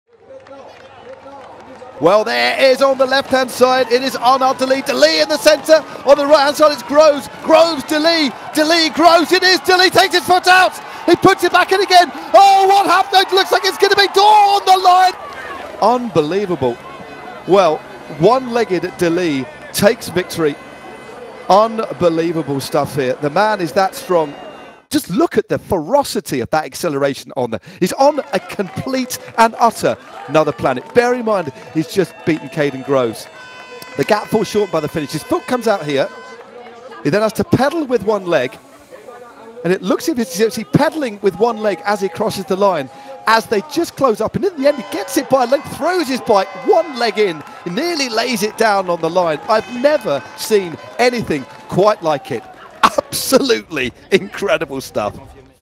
Who Needs Two Legs Anyway??
What a finish!

Arnaud De Lie held on to win the Lotto Famenne Ardenne Classic despite unclipping before the line!

Watch the race replay over on GCN+ 👉 gcn.eu/racetv

Useful Links:
Everything Cycling. All In One Place